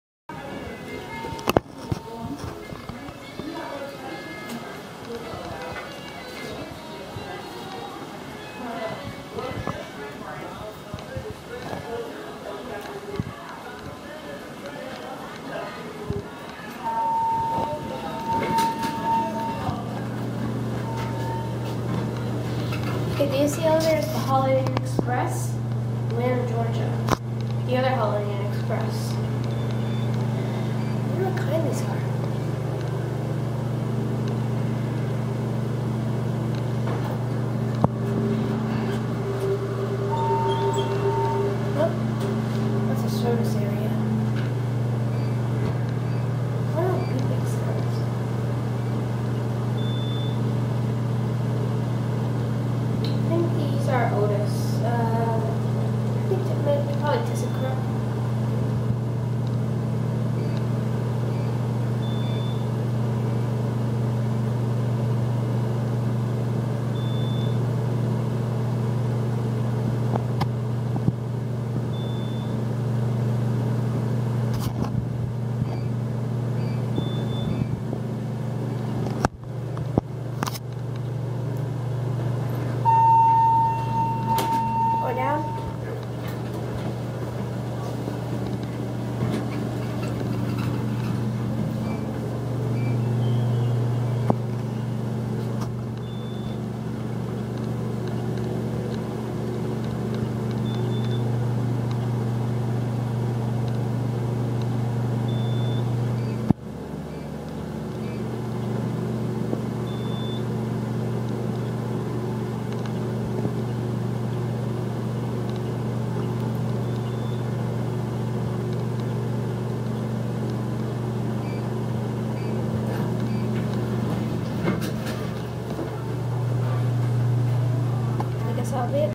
Elevators at the Holiday inn Express in Midtown Atlanta GA
These are the elevators at the Holiday Inn express in Midtown Atlanta GA.  The elevators are Montgomery and serve 7 floors.